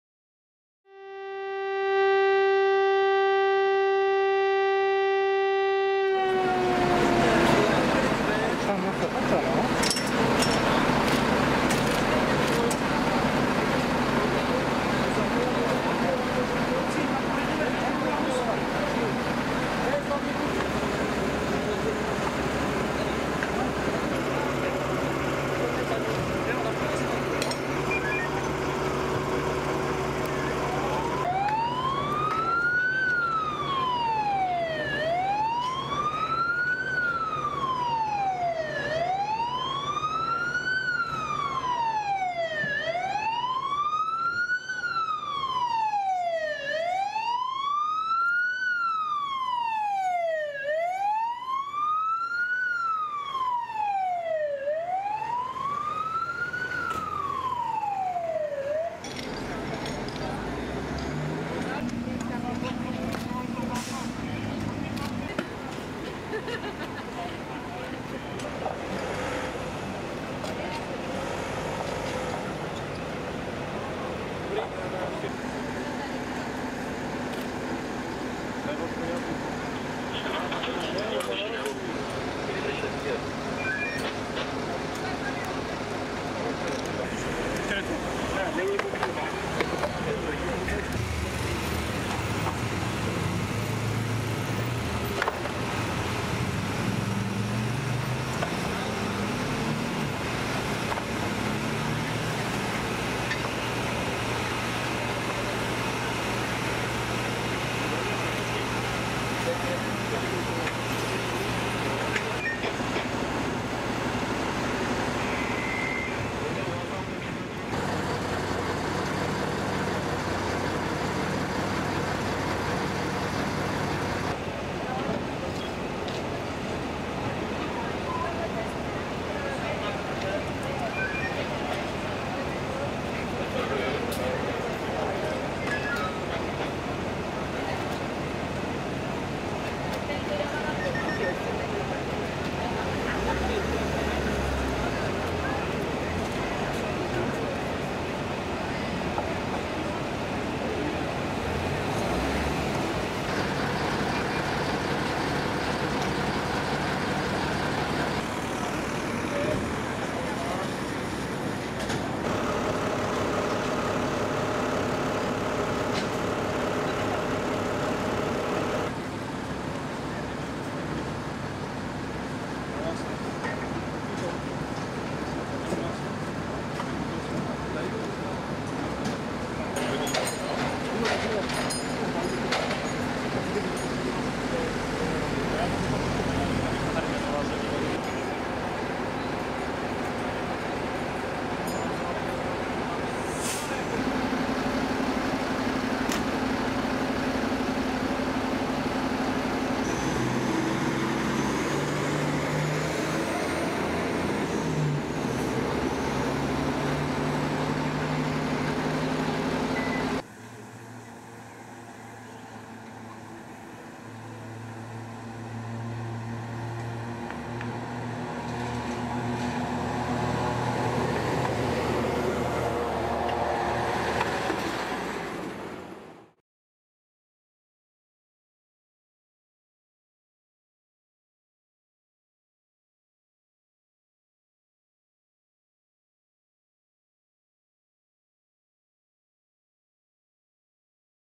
Přísnotice - Požár v RD
Požár v podkroví RD v Přísnoticích, 24. června 2012.